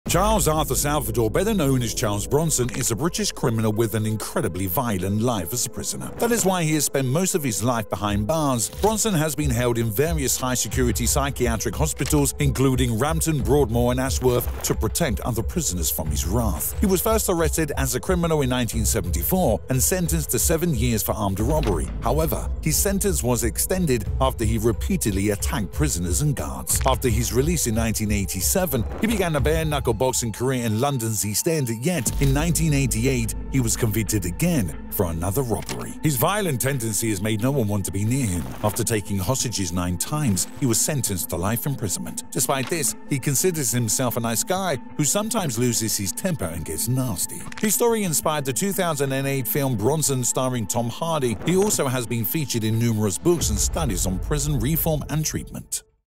This Man Was The Most FEARED Man In PRISON?? 😱💪🏼
In this video, we explore the fascinating story of Charles Arthur Salvador, also known as Charles Bronson, the "Most Feared Man in Prison". From his early life struggles to his notorious reputation in the British prison system, we delve into the reasons behind his violent outbursts and the impact they had on his life and those around him. Join us on this journey to discover the truth about this infamous prisoner and the legacy he has left behind.

This channel neither endorses nor promotes the criminal acts depicted in these videos. The individuals showcased are offenders who should face legal consequences.